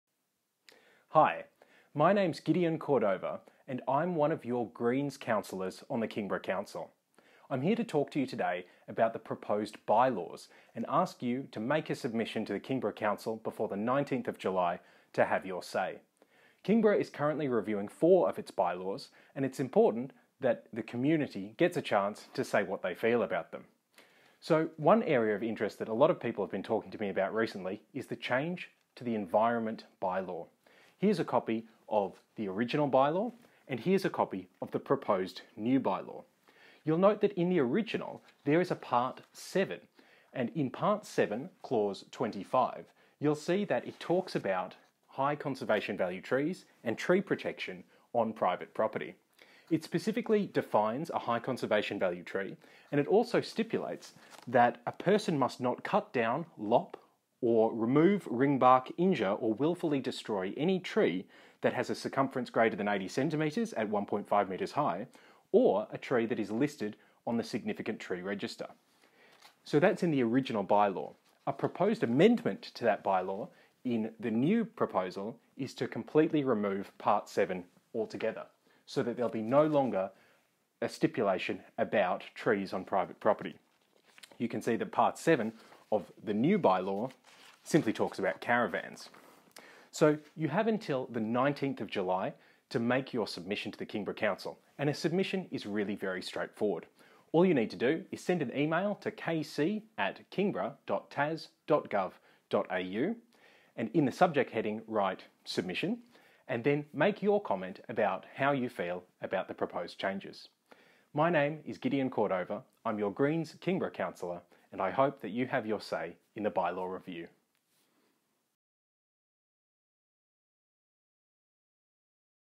Kingborough By-Law Review: Submissions close 19 July 2021!!
Kingborough's Environmental By-Law is up for review. The proposed changes include removing Part 7 of the Environmental By-Law which talks about trees on private property and requires the payment of "offset" financial contributions for those who seek to damage significant trees or trees of high conservation value.

TO MAKE YOUR SUBMISSION TO KINGBOROUGH COUNCIL:
Step 1: review the proposed by-laws on the Kingborough Council website
Step 3: write your comments about the proposed changes to the by-laws
You can also send your submission via post to Kingborough Civic Centre, 15 Channel Hwy, Kingston, Tasmania, 7050 or submit it in person at the Civic Centre. Submissions can be made up to the close of business on 19th of July 2021.

The public has until the 19th of July, 2021 to make a submission to the Kingborough Council to let them know whether or not you support the changes. More information about the public consultation is available here:

The proposed changes to the Environmental By-Law includes removing the following:

PART 7: TREES ON PRIVATE PROPERTY.
Removal of trees on private property
25 - (1) In this Part -

"high conservation value tree" means a tree that is of a species that is listed in the Threatened Species Protection Act 1995 or the Environment Protection and Biodiversity Conservation Act 1999 (Cth) and/or which provide habitat for a threatened species listed in either of those acts.

"offset" means an offset under the Council's Policy 6.10 Biodiversity Offset Policy as amended from time to time, or in the absence of any current Council policy, an action the Council requires a  person to take that aims to compensate for an environmental impact that results in a loss of biodiversity caused by the person. An offset can include active management, a financial contribution and other actions that demonstrate a conservation benefit for a particular natural value.

(2)        A person must not cut down, top, lop, remove, ringbark, injure or wilfully destroy any tree which:

(a)         has a trunk circumference of greater than 80cm at 1.5 m or more above ground level, unless authorised by a permit to do  so; or

(b)         is listed on a register of significant trees applicable to the municipal area, unless authorised by a permit to do so; or,

(c)         is protected under an agreement under Part 5 of the Land Use Planning and Approvals Act 1993 or covenant on the title.

Penalty:      Fine not exceeding 15 penalty units

(3)        Sub-clause (2) does not apply to:

(a)         an environmental weed as listed in a planning  scheme  applicable to the land on which the environmental weed is located;

(b)         a declared weed under relevant State weed legislation;

(c)         an exotic species (including non-Tasmanian natives) not listed  on a register of significant trees applicable to the municipal area; or,

(d)         a tree whose removal is approved under a permit in respect of a use or development that is issued under the Land Use Planning and Approvals  Act  1993.

(4)        Where a permit is granted under sub-clause (2) the Council may require an offset if the tree that is the subject of the permit is listed on a register of significant trees applicable to the municipal area or is a high conservation value tree.

I encourage everyone to take advantage of the public consultation period to get a submission in before close of business 19 July 2021. The full report can be read on Council’s website in the agenda for the Council meeting held on 7 June 2021. The report outlines at 4.5 there have been numerous updates to the existing By-laws to reflect changes in legislation and contemporary community expectations.  This includes the removal of provisions relating to the regulation of native trees on private land from the Health and Environmental Services By-law following legal advice.